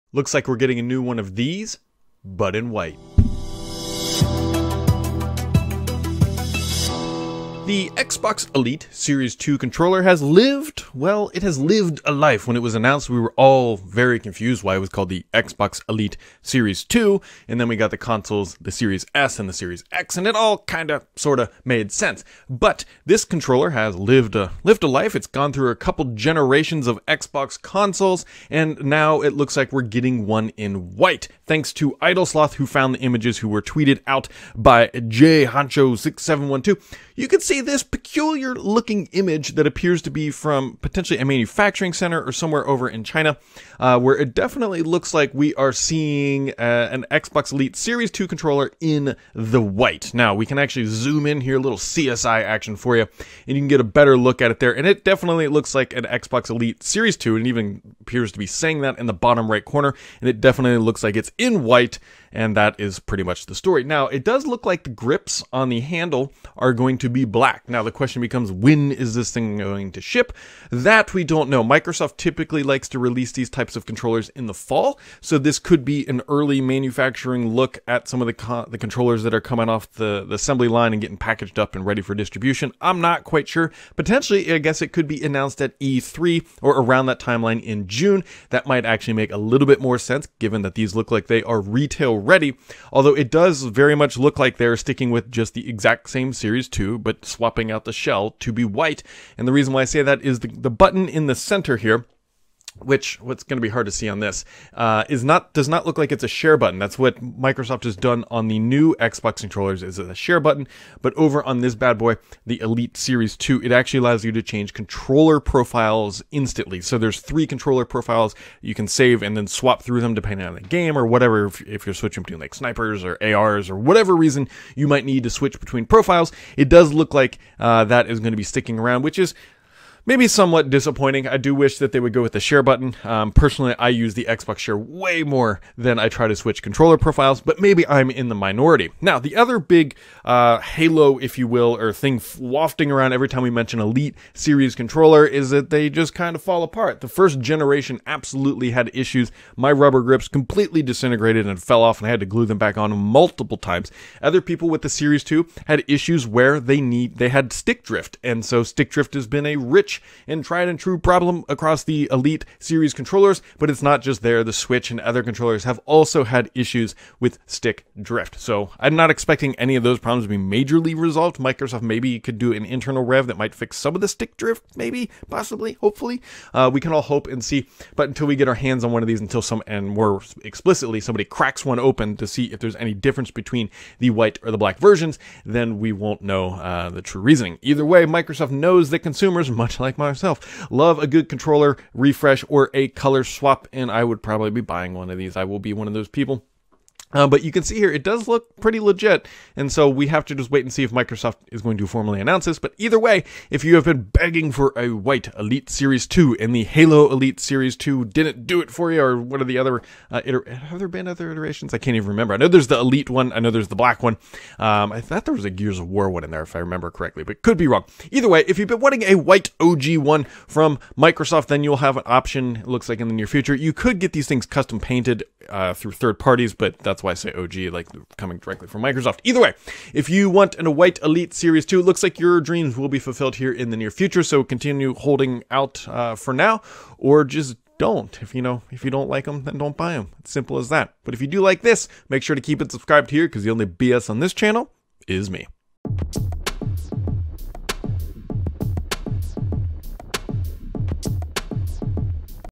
An Xbox leak in White
Looking to pick up a new Elite controller? Looks like you will soon be able to grab one in white.